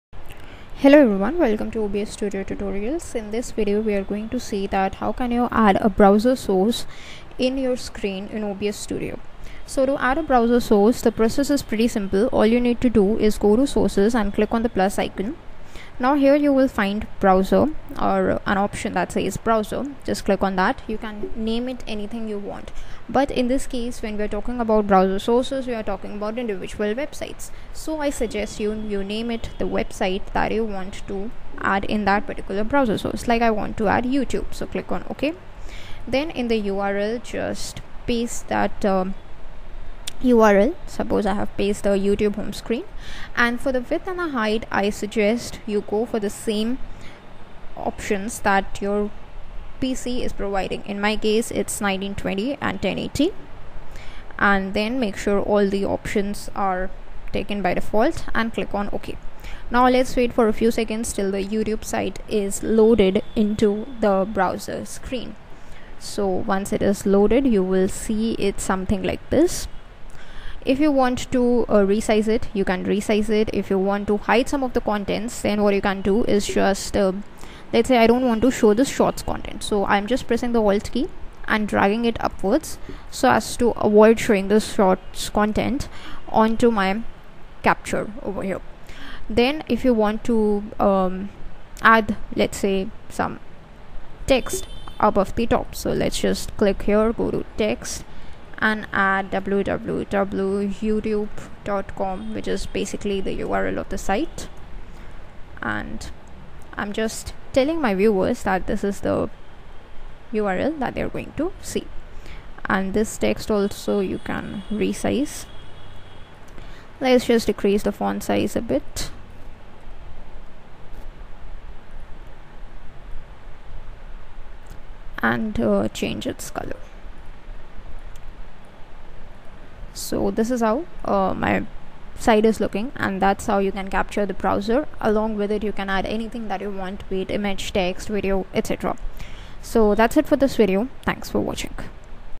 How to Add Browser Source to OBS Studio
Title: How to Add Browser Source to OBS Studio | Integrate Web Content Into Your Streams

Description:
Enhance your live streams by learning how to add a browser source to OBS Studio. In this tutorial, we'll guide you through the steps to seamlessly integrate web content, overlays, alerts, and more directly into your OBS Studio scenes. Whether you're a gamer, content creator, or live streamer, this quick and easy tutorial will empower you to showcase dynamic web content and engage your audience like never before.

### Key Steps Covered in This Tutorial:
1. **Open OBS Studio:**
   - Launch OBS Studio on your computer. If you don't have it installed, download and install OBS Studio from the official website.

2. **Go to the Scene:**
   - Select the scene where you want to add a browser source or create a new scene.

3. **Add a Source:**
   - In the Sources section, click on the "+" button and select "Browser" from the menu.

4. **Name the Source:**
   - Give your browser source a descriptive name, such as "Web Overlay" or "Alerts."

5. **Configure Browser Source Settings:**
   - In the Browser Source properties, enter the URL of the web content you want to display. Adjust width, height, and other settings as needed.

6. **Test the Browser Source:**
   - Click "OK" or "Apply" to add the browser source to your scene. You can resize and reposition the source as necessary. Test the source to ensure it displays correctly.

7. **Done! Browser Source Added to Scene:**
   - Once satisfied with the configuration, your browser source is ready to be used in your OBS Studio scene.

### Benefits of Adding Browser Source to OBS Studio:
- **Dynamic Overlays:** Incorporate dynamic overlays, animations, and alerts into your streams.

- **Live Web Content:** Showcase live web pages, social media feeds, and more during your broadcasts.

- **Engaging Interactivity:** Enhance audience engagement with real-time web elements.

Follow along with this tutorial to effectively add a browser source to your OBS Studio scenes and elevate your streaming experience with dynamic web content.